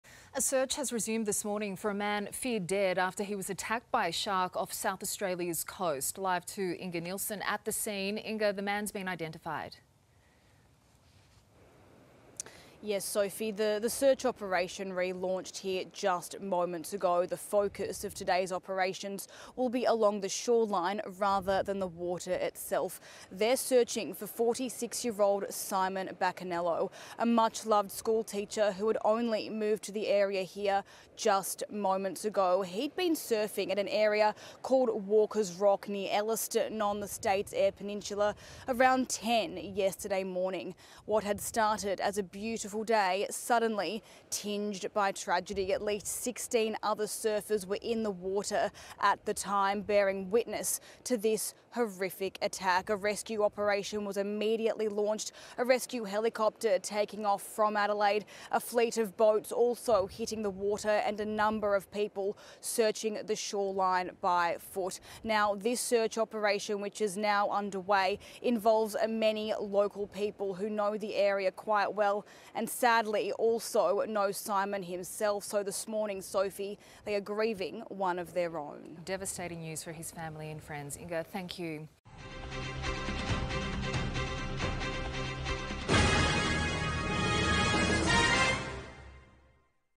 Search to resume for surfer believed dead after SA shark attack | 9 News Australia
46-year-old Simon Baccanello, is believed to have been killed in a shark attack off a beach in South Australia, in marking the state's first such death since 2014.